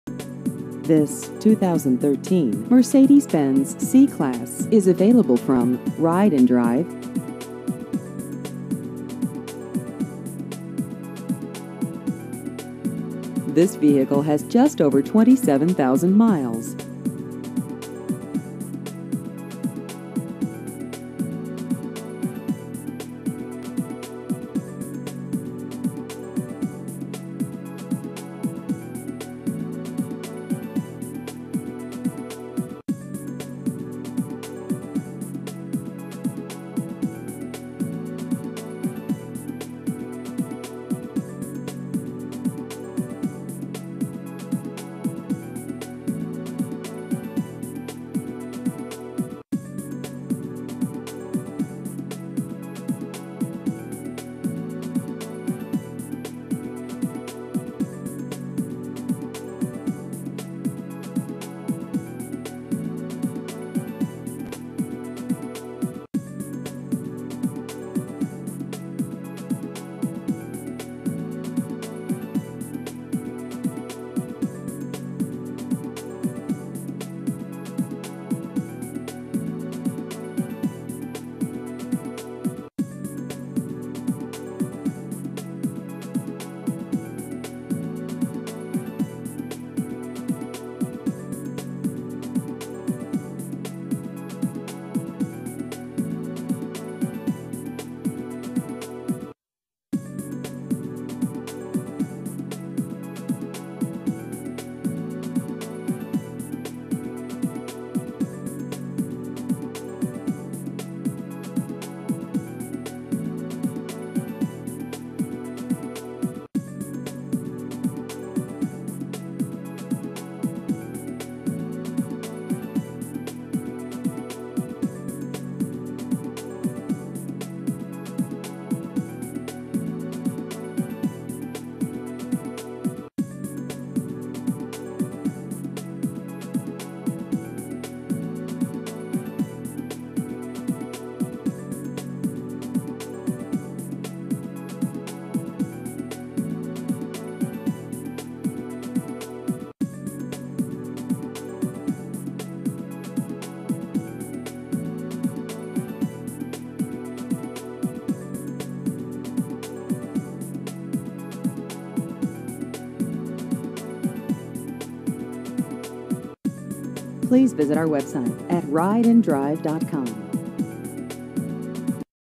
2013 Mercedes-Benz C-Class Used Cars Fishers IN
This 2013 Mercedes-Benz C-Class is available from Unlimited Motors.